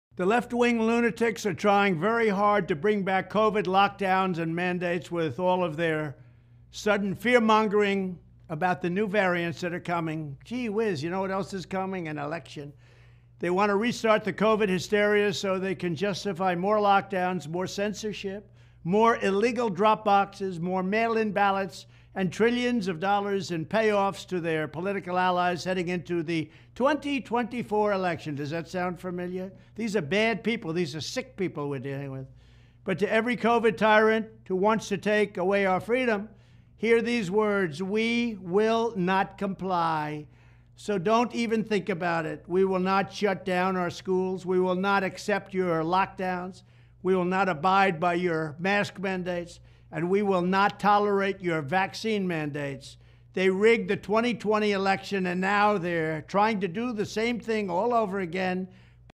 🚨President Donald Trump! 🚨 The Radical Left is becoming increasingly desperate! MonkeyPox ☢️
🚨 Important Message from President Donald Trump! 🚨 The Radical Left is becoming increasingly desperate! 🚨

“In a time of deceit telling the truth is a revolutionary act.”
George Orwell